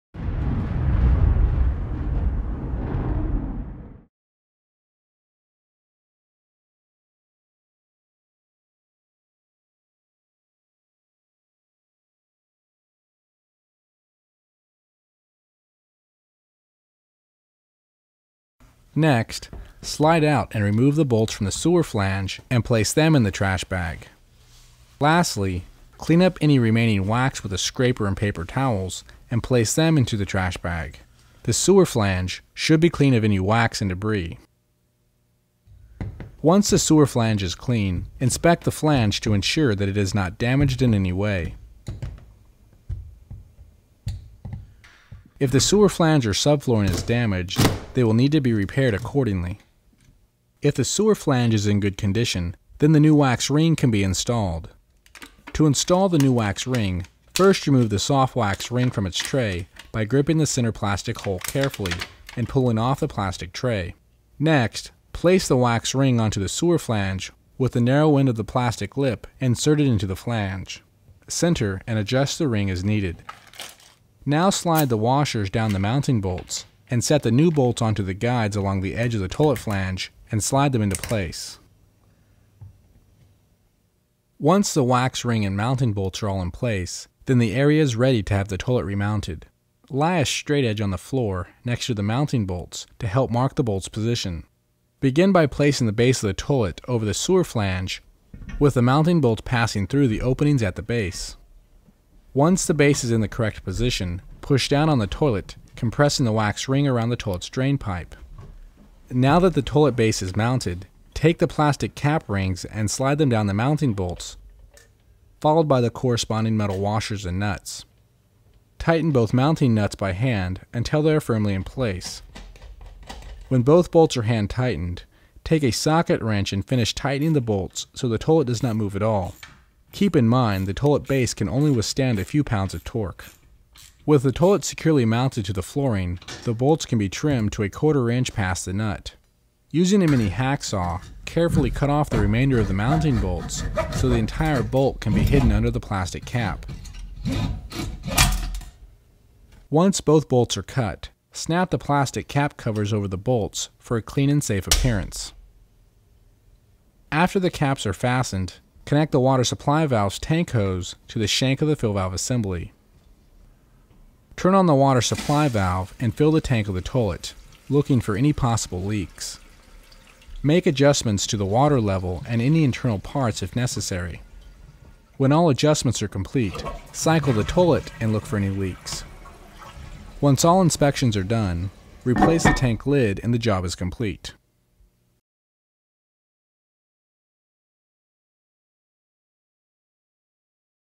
Toilet Flange Wax Ring Replacement Part 2 of 2
Next, slide out and remove the bolts from the sewer flange and place them in the trash bag.  Lastly, clean up any remaining wax with the scraper and paper towels and place them into the trash bag.  The sewer flange should be clean of any wax and debris.

Once the sewer flange is clean, inspect the flange to ensure that it is not damaged in any way.  If the sewer flange or sub-flooring is damaged, they will need to be repaired accordingly.

Wax Ring Replacement

If the sewer flange is in good condition then the new wax ring can be installed.  To install the new wax ring first remove the soft wax ring from its tray by griping the center plastic hole carefully, and pulling off the plastic tray.  Next, place the wax ring onto the sewer flange with the narrow end of the plastic lip inserted into the flange.  Center and adjust the ring as needed.  Now slide the plastic holding washers down the mounting bolts and set the new bolts into the mounting channels along the edge of the toilet flange and slide them into place.

Toilet Replacement

Once the wax ring and mounting bolts are all in place then the area is ready to have the toilet remounted.  Lay a straight edge on the floor next to the mounting bolts to help mark the bolts position.  Begin by placing the base of the toilet over the sewer flange with the mounting bolts passing through the openings at the base.  Once the base is in the correct position push down on the toilet compressing the wax ring around the toilet's drainpipe.

Now that the toilet base is mounted, take the plastic cap rings and slide them down the mounting bolts, followed by the corresponding metal washers and nuts.  Tighten both mounting nuts by hand until they are firmly in place.  When both bolts are hand tightened, take a socket wrench and finish tightening the bolts so the toilet does not move at all.  Keep in mind the toilet's base can only withstand a few pounds of torque.
With the toilet securely mounted to the flooring the bolts can be trimmed to a quarter-inch past the nut.  Using a mini-hacksaw carefully cut off the remainder of the mounting bolts so the entire bolt can be hidden under the plastic cap.  Once both bolts are cut, snap the plastic cap cover over the bolts for a clean safe appearance.

After the caps are fastened, connect the water supply valve's tanks hose to the shank of the fill assembly.  Turn on the water supply valve and fill the tank of the toilet, looking for any possible leaks.  Make adjustments to the water level and any internal parts if necessary.  When all adjustments are complete, cycle the toilet and look for any leaks.  Once all inspections are done replace the tank lid and the job is complete.